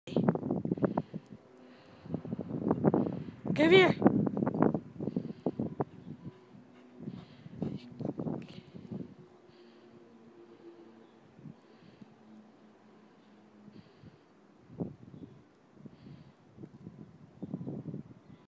Happy Jack
I love seeing how happy this dog is when he gets to exercise.